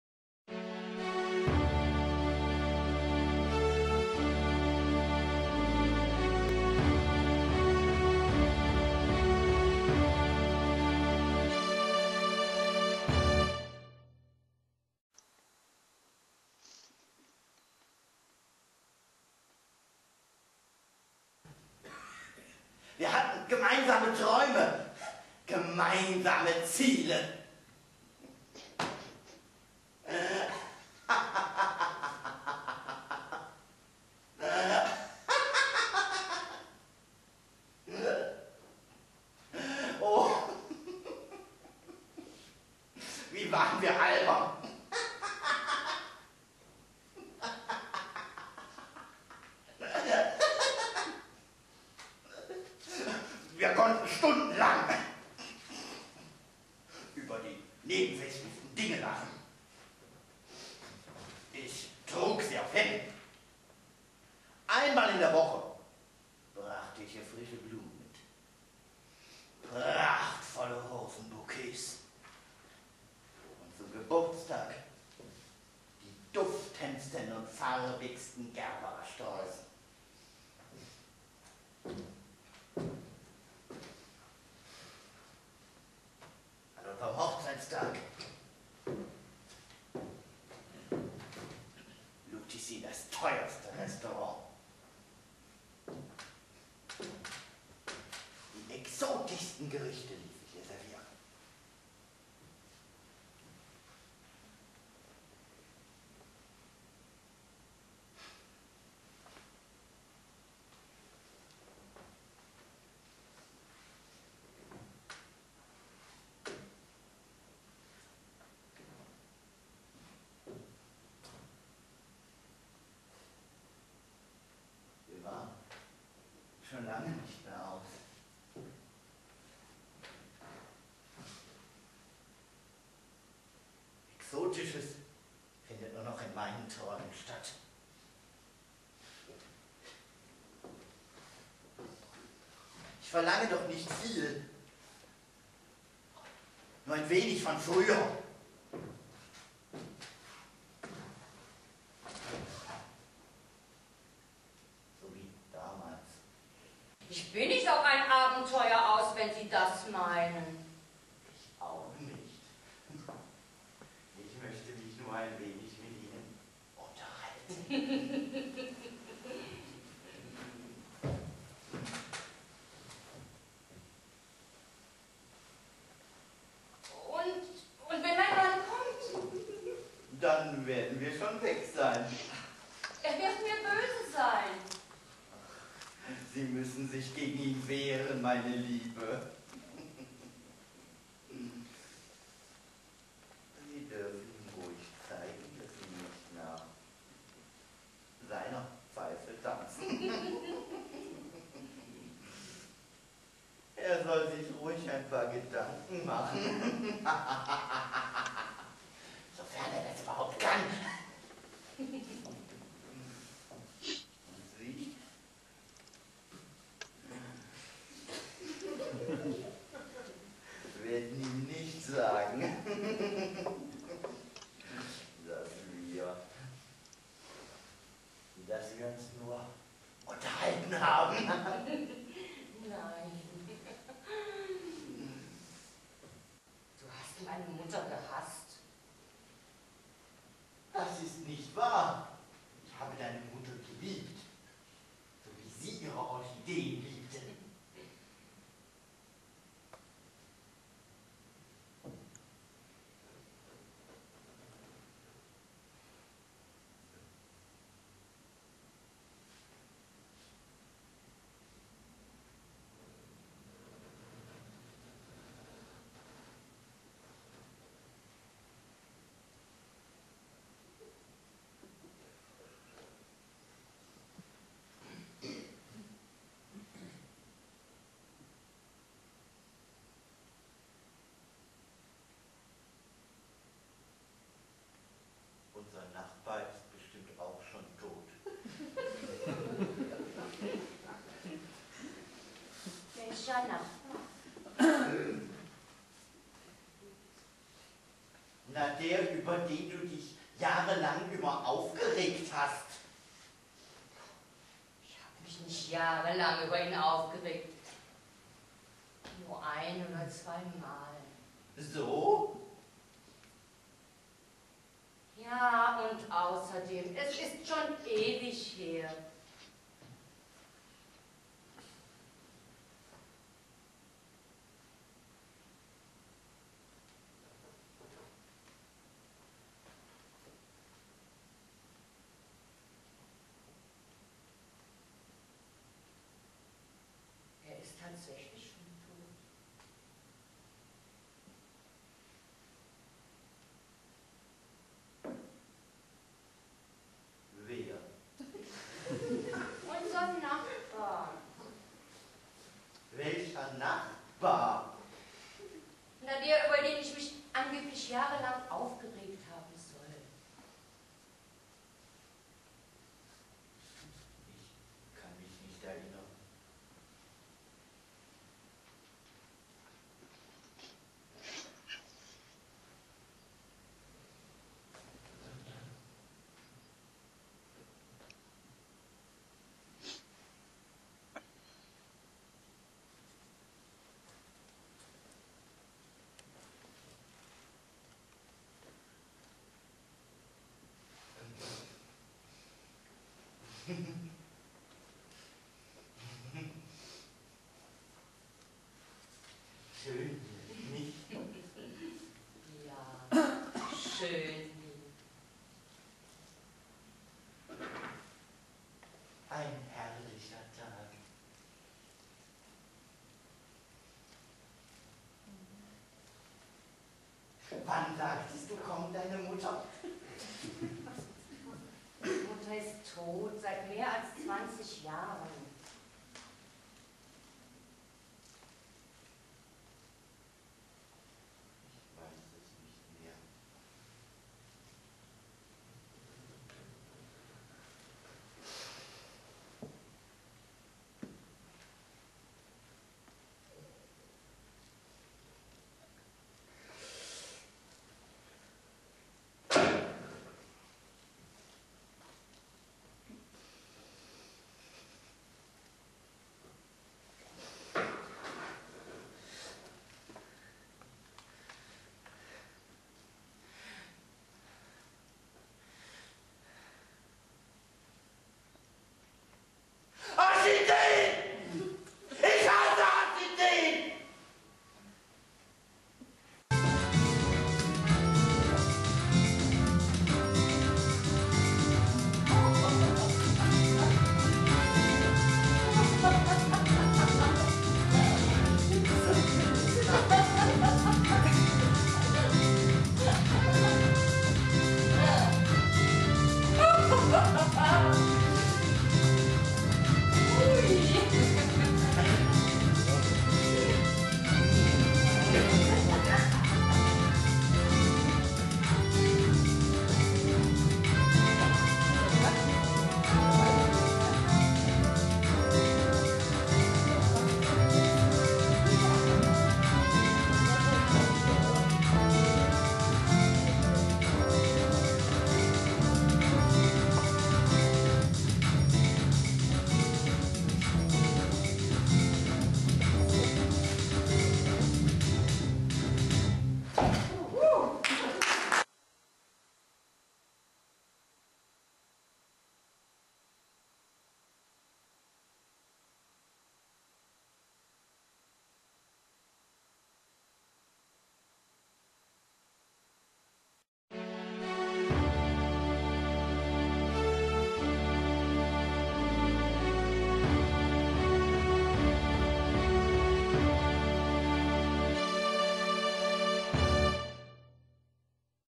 Ausschnitte aus Cello oder Du und ich von Hans Peter Zeller
SIE und ER, ein Ehepaar, durchleben verschiedene Statio­nen ihres Lebens und geben Einblick in ihre Gedanken von einst, heute und morgen. Typische "Ehealltagsszenen" werden dargestellt: Das Frühstück mit Zeitung und Ruhebedürfnis einerseits, Ge­sprächsbedürfnis und das Gefühl des Vernachlässigtwer­dens auf der anderen Seite. Die gleichen Szenen werden mehrmals durchgespielt - mit vertauschten Rollen, am An­fang der Ehe und im Alter, oder unter dem Gesichtspunkt: "Was hätte geschehen können, wenn ich mich anders ver­halten hät­te". Die verschiedenen Stationen werden dargestellt am Früh­stückstisch, in einer Bar und im Park eines Senioren­heims.